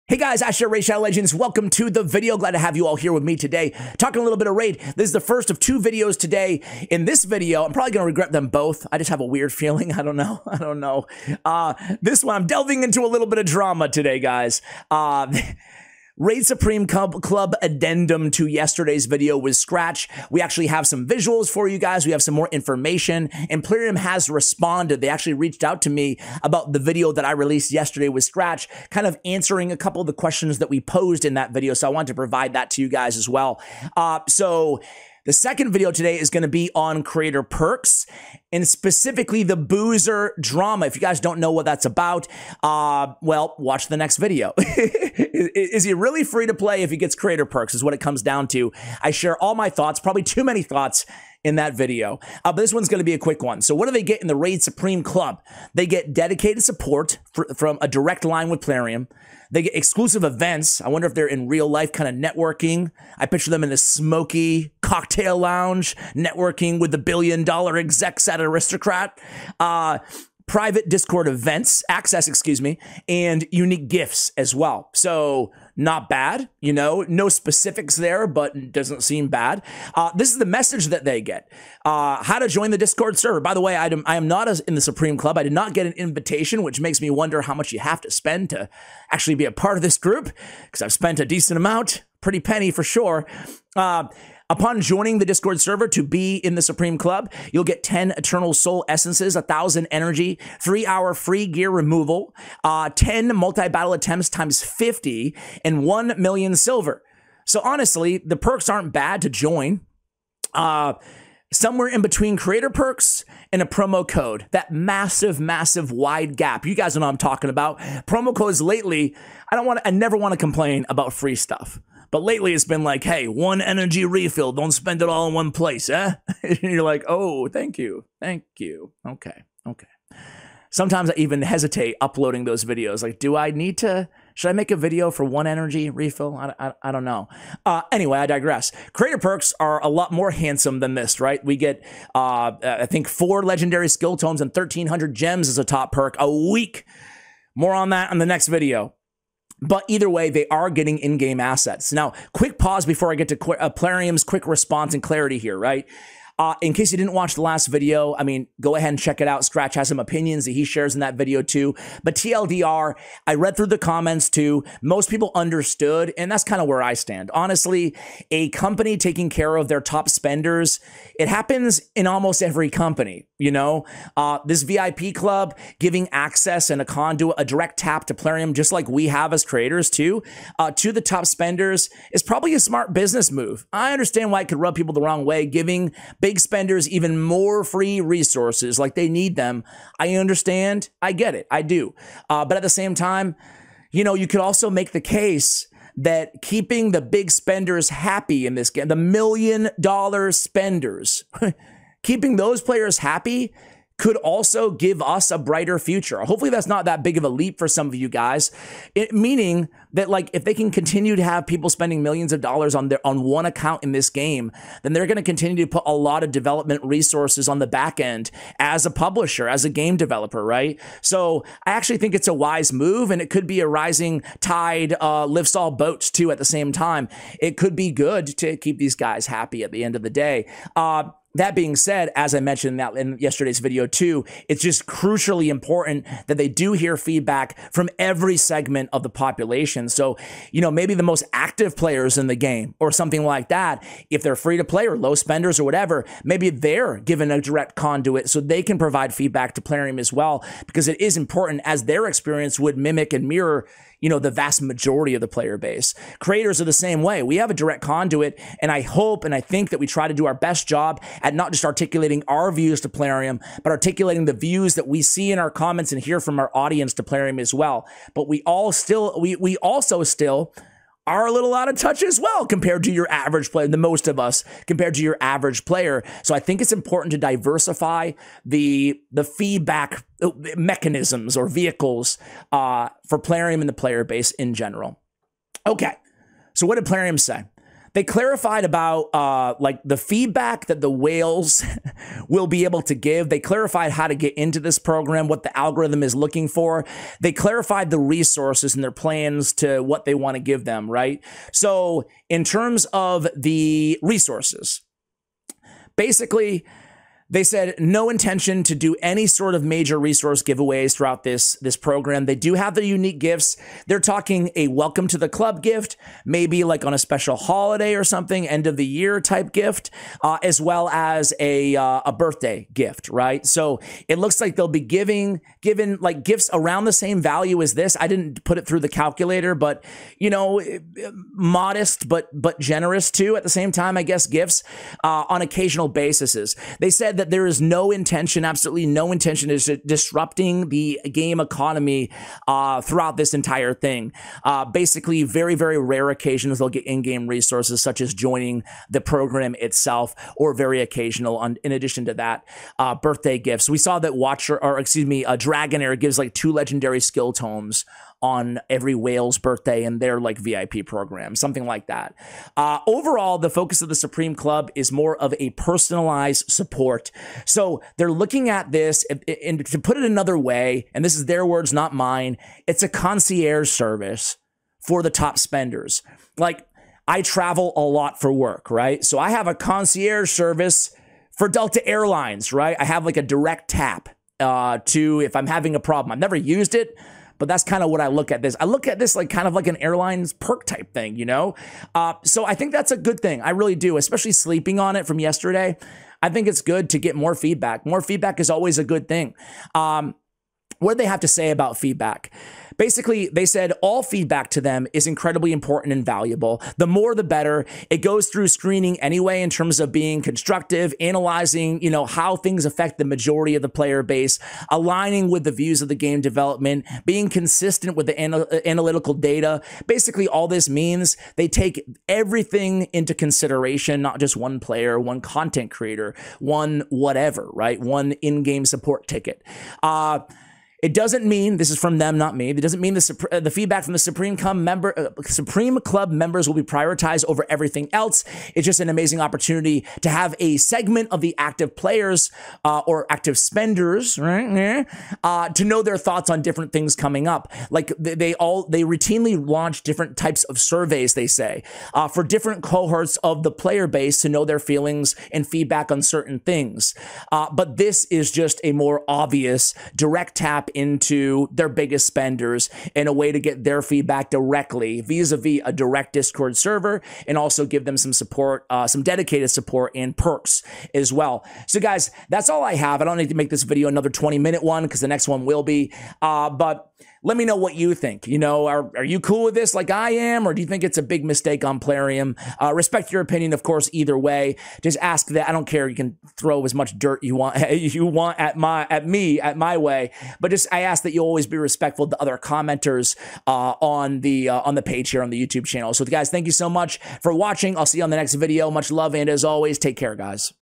PLARIUM RESPONDS to "SUPREME CLUB" CONCERNS!
RAID Shadow Legends | Plarium Responds to Supreme Club

Today in RAID we'll talk about the Supreme Club and Plarium's response! Enjoy!

Social Media-
►►Facebook - Clash With Ash Official